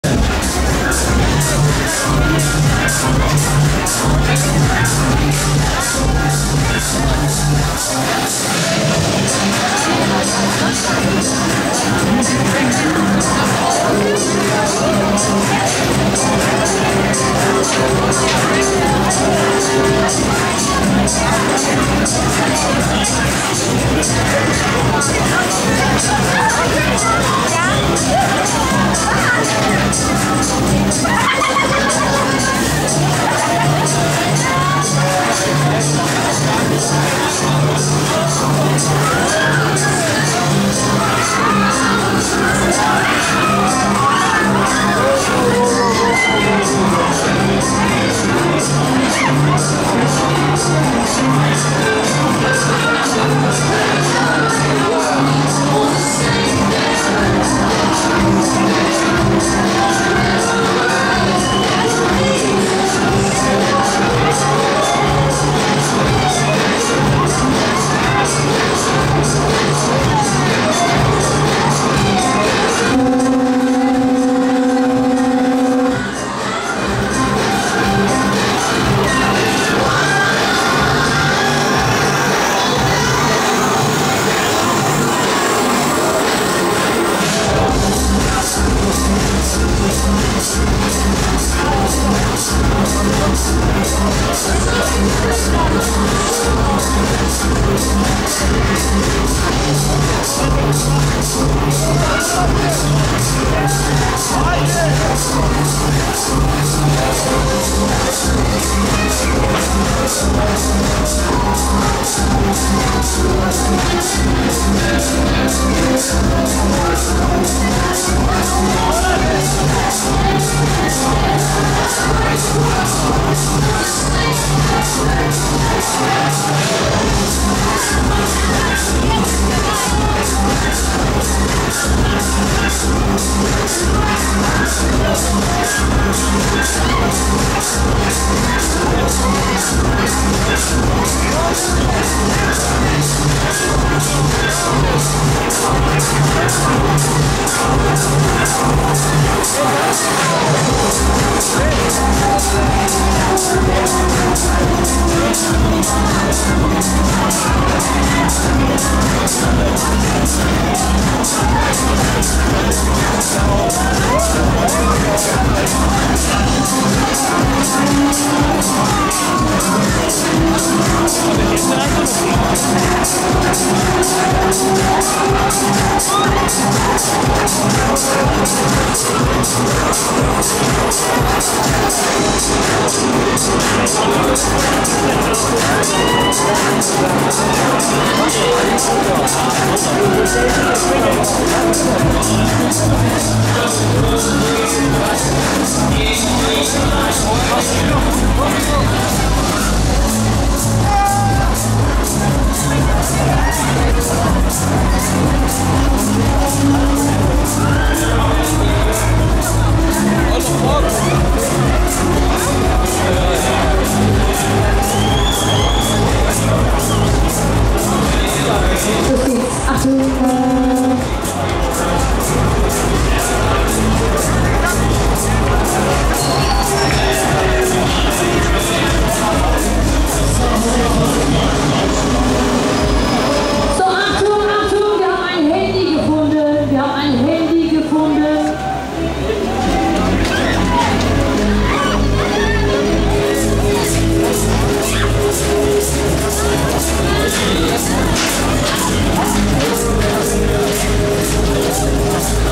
Break Dance (Klinge) - Außenansicht/Offride 2
Das Video wurde auf dem Kölner Herbstvolksfest 2016 aufgenommen. Bei diesem Fahrgeschäft handelt es sich um einen Huss Break Dance.